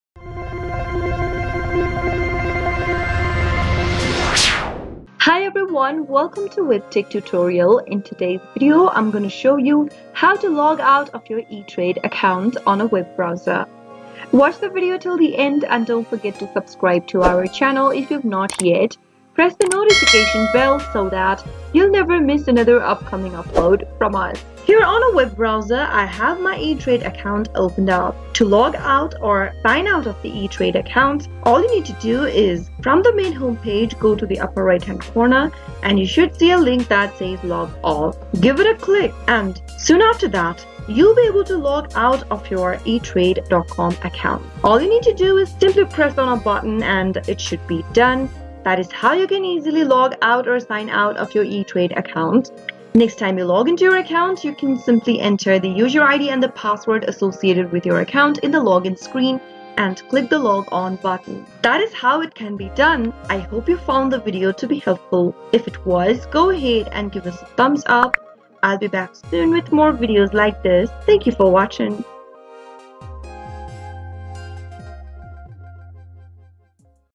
eTrade Logout: How to Sign Out Logout of Etrade Account 2022?
How to Logout of Etrade | Sign-Out Etrade Account

Confused about how to logout of Etrade? In this video, we'll learn the exact steps on how to logout of Etrade. All you need to do is go to the upper right hand corner and you should see a link that says log all. Give it a click and soon after that, you'll be able to log out of your e-trade.com account.